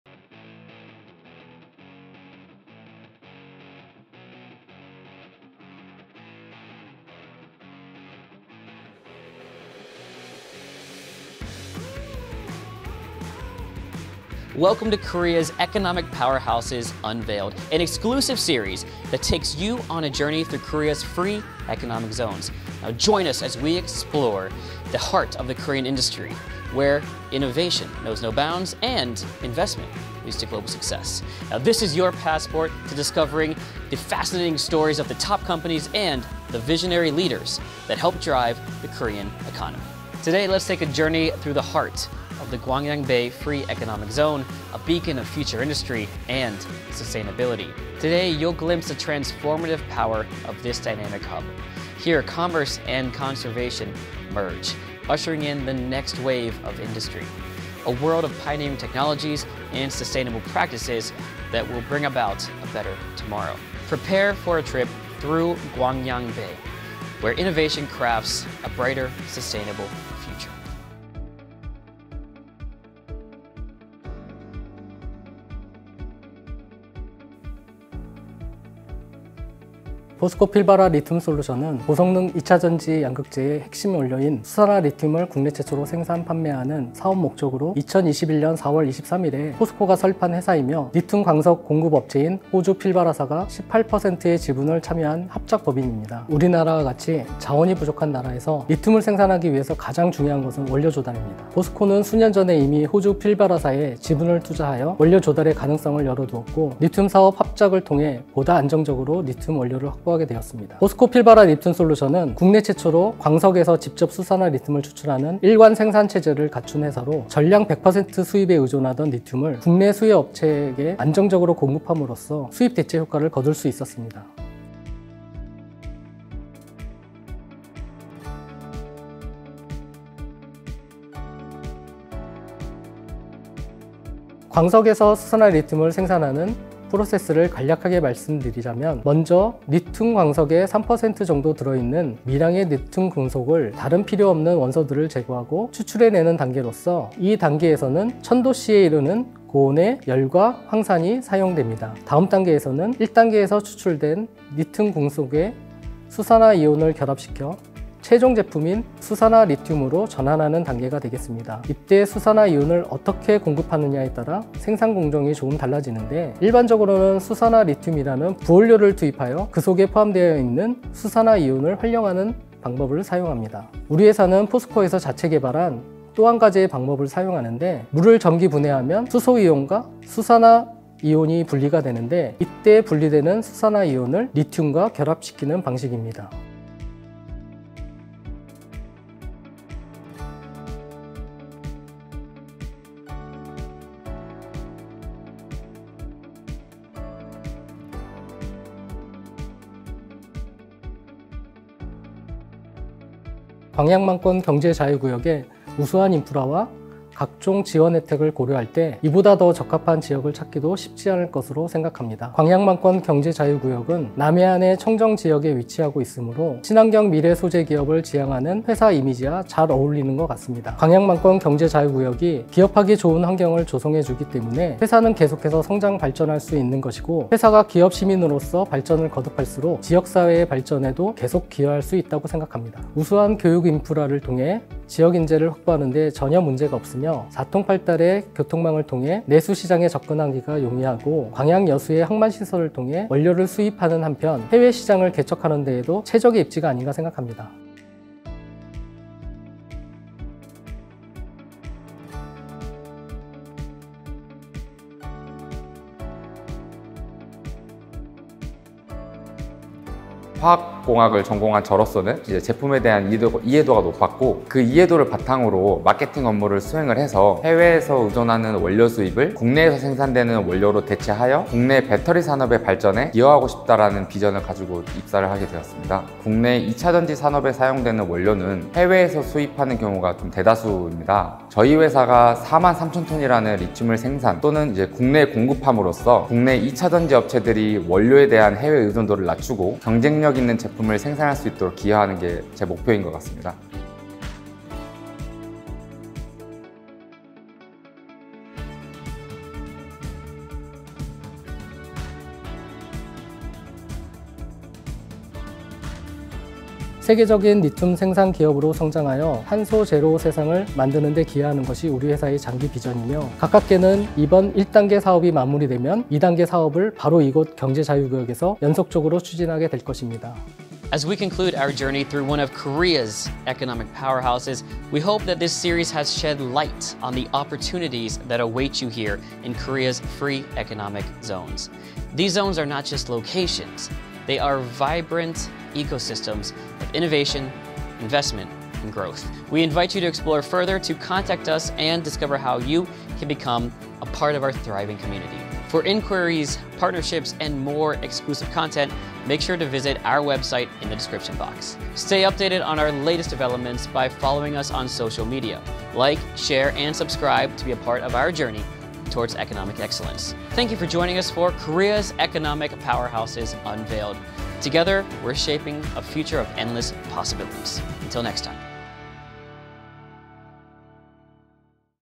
광양만권경제자유구역 우수입주기업 인터뷰🎥 / 포스코필바라리튬솔루션🌏🌱
광양만권경제자유구역에 위치하여
친환경 미래 소재 산업의 글로벌 리더로 도약하는
포스코필바라리튬솔루션의 인터뷰🌏🌱
📌 01:20 포스코필바라리튬솔루션 소개
📌 02:14 광석에서 수산화리튬을 생산하는 프로세스
📌 03:23 광양만권경제자유구역의 장점
📌 04:20 입사 동기와 앞으로의 비전
📌 05:06 포스코필바라리튬솔루션의 장기 비전
광양만권경제자유구역청 바로가기
경제자유구역 기획단 홈페이지 바로가기
대한민국경제자유구역 SNS 채널